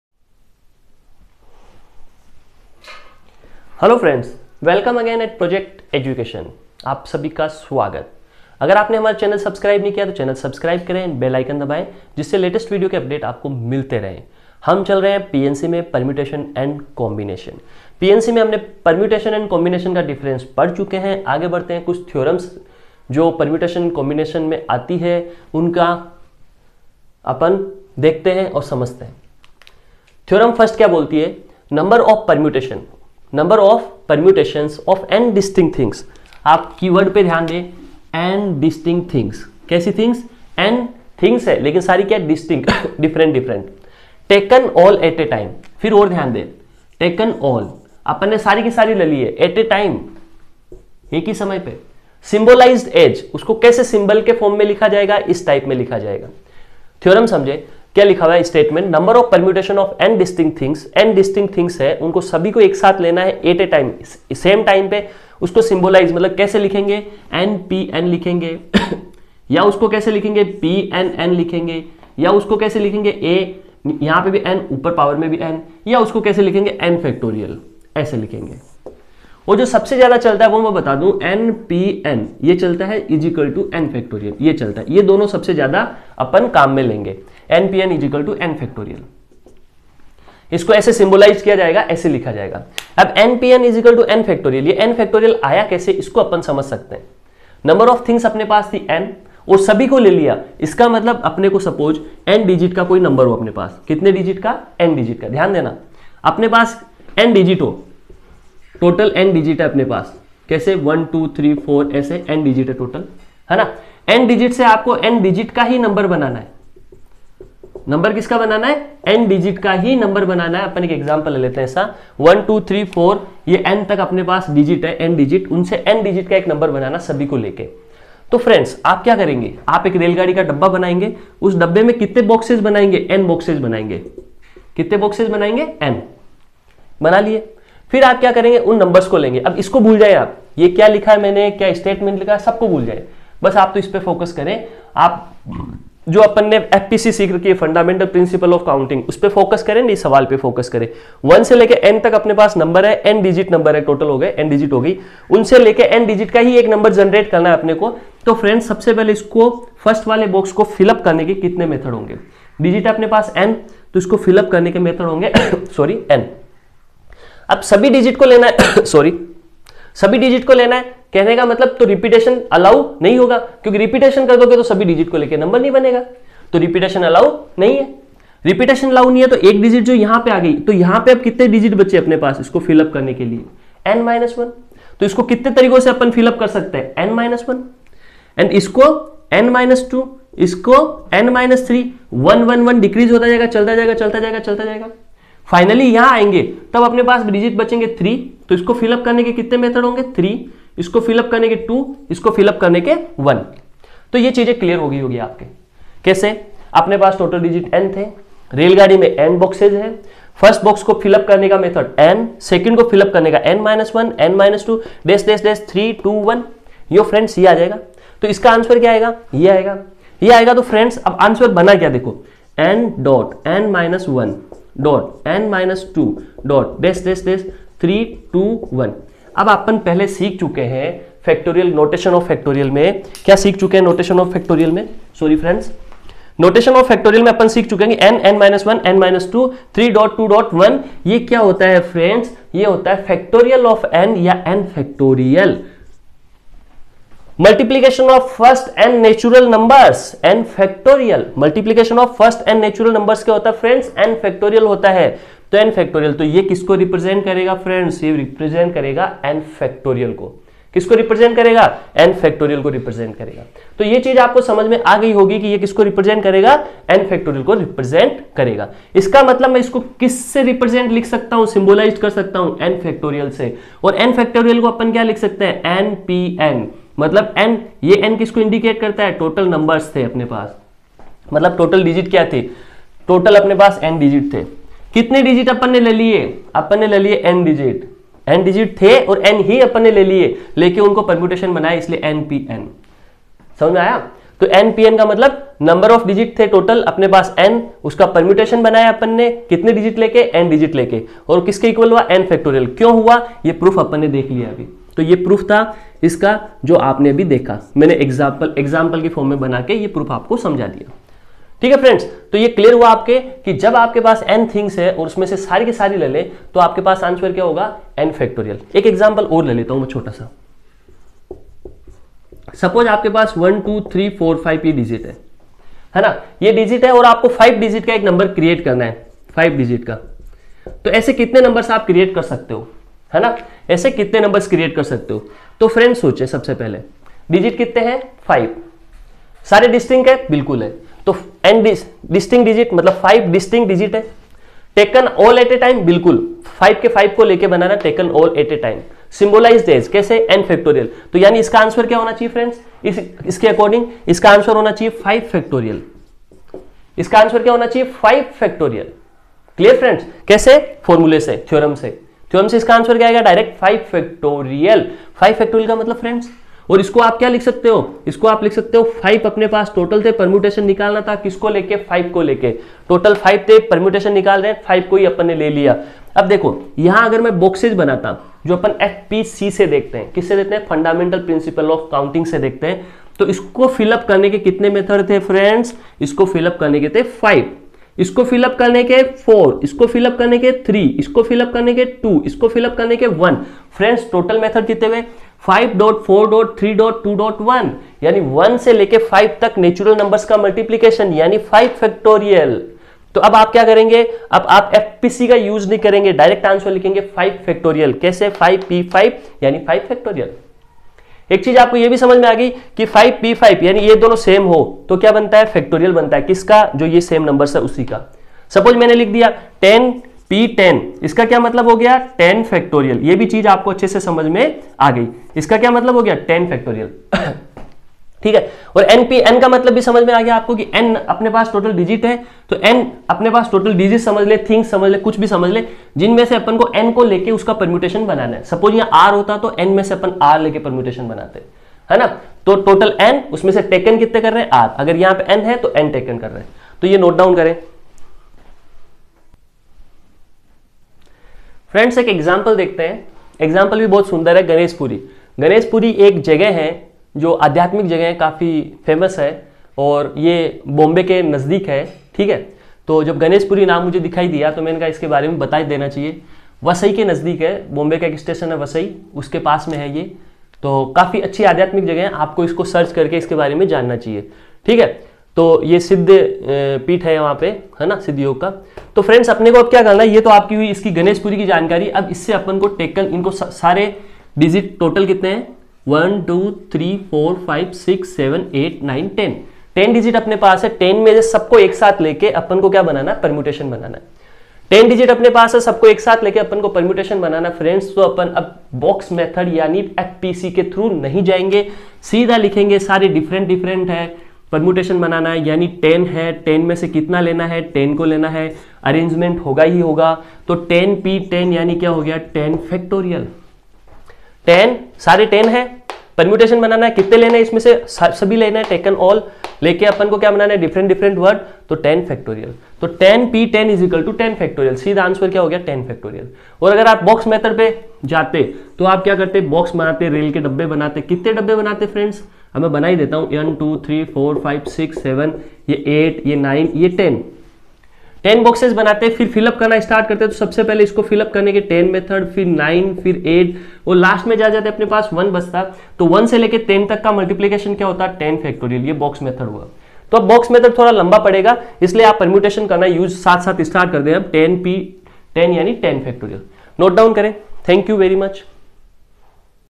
Permutations and combinations part-9a | nPn theorem and derivation with proof
The number of permutations of n distinct things taken all at a time is defined by nPn.

Derivation of nPn theorem of permutations and combinations.

If n digits are given and all n digits are to be taken at the same time and no repetition is allowed then number of ways these n digits can be arranged is nPn.
The value of nPn is equal to n!

Understanding nPn theorem is very important. This theorem of often used to solve permutations and combinations problems. This theorem comes in handy and will save lots of time in IIT JEE Mains and Advanced exams.

You can either solve by box method or nPn method. nPn method i.e. permutations method is relatively fast.

We provide completely free videos for IIT JEE Maths and other subjects. You can ask your doubts in the comments.